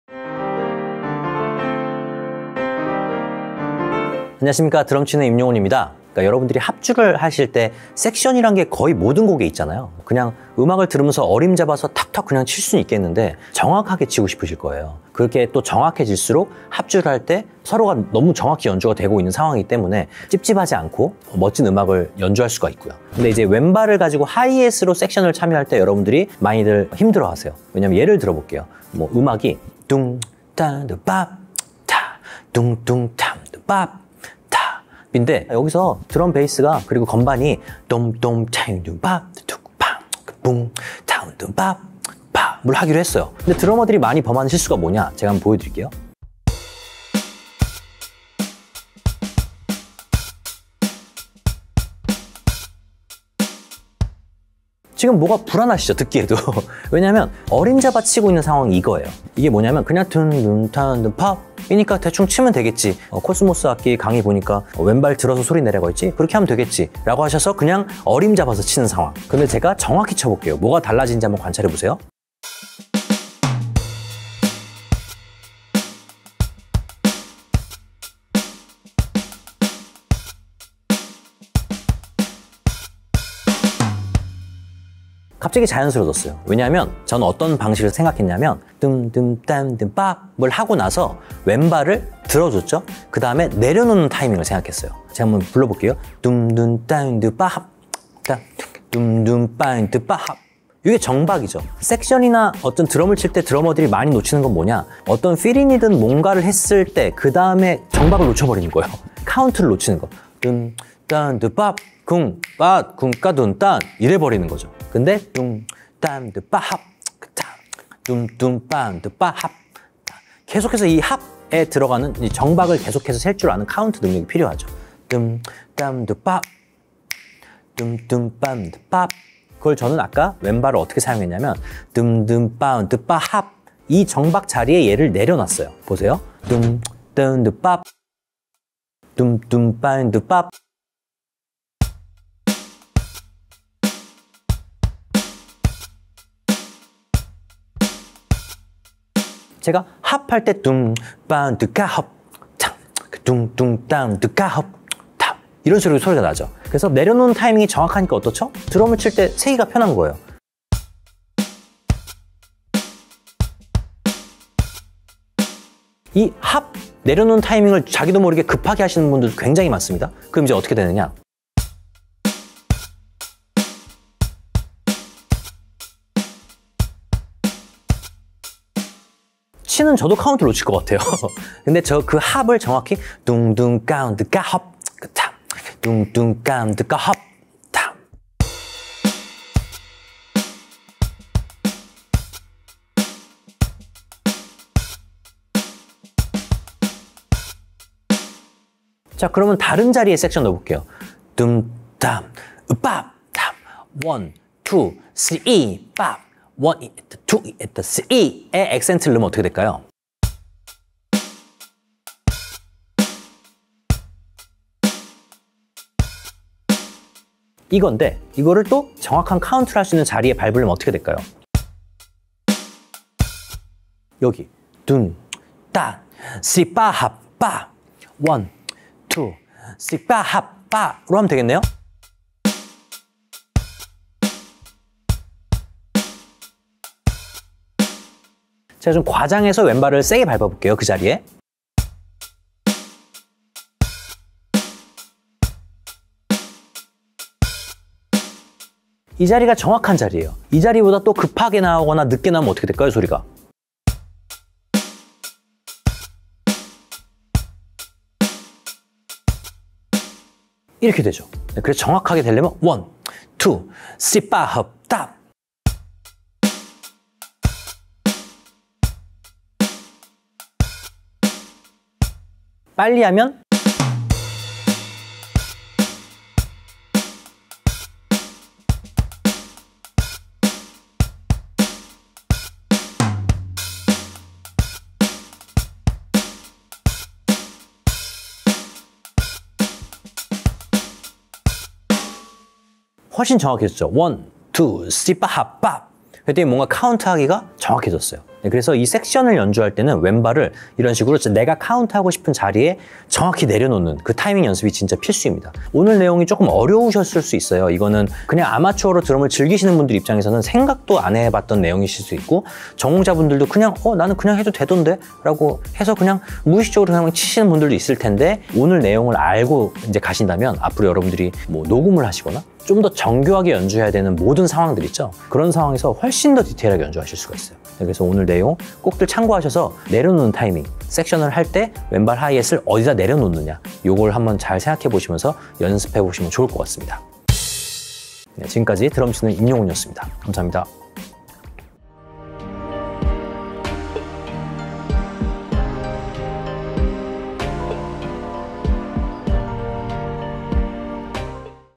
이거 신경 안 쓰면 어디 가서 초보 소리 듣는다...왼발 하이햇 섹션 꿀팁 ! I 롤랜드 VAD706 드러머 임용훈 강좌
최고의 드럼 테크니션
임용훈 선생님이 알려주는 드럼 꿀팁!

신경쓰지 않으면 연주의 디테일이 크게 떨어질 수 있는
왼발 하이햇 섹션 꿀팁에 대해서 배워보는 시간을 가져보겠습니다.

【 사용 악기 】

Roland VAD706

【 타임 테이블 】

00:00 드럼 섹션?
00:23 왼발 하이햇 섹션할 때 자주 하는 실수
01:26 왼발 하이햇 섹션 꿀팁
05:05 섹션에 대해서 하고 싶은 말
05:38 클로징